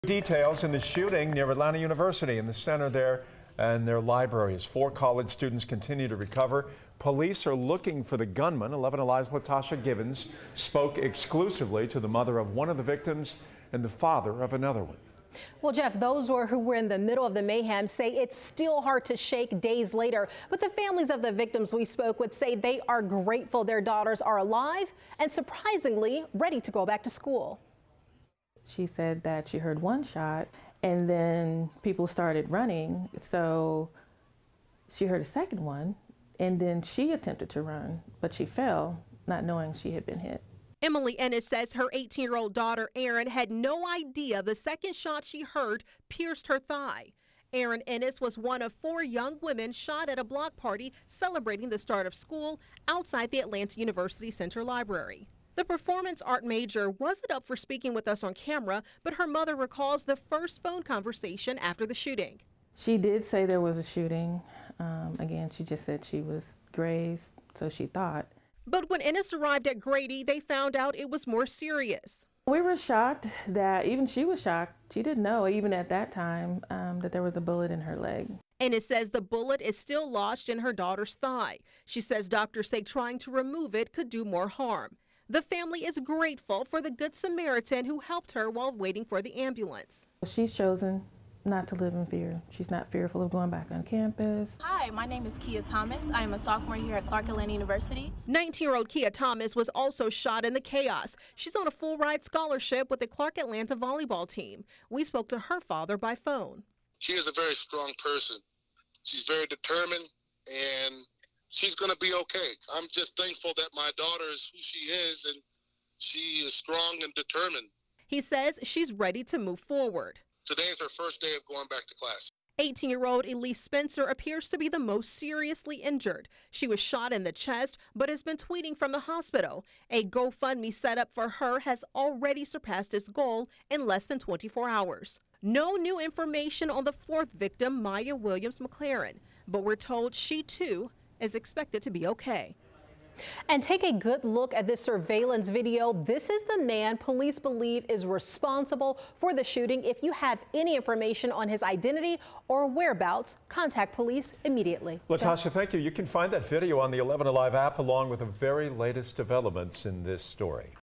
Families of students hurt in shooting outside AUC library speak
The families are grateful their daughters are alive. Here's what else they had to say.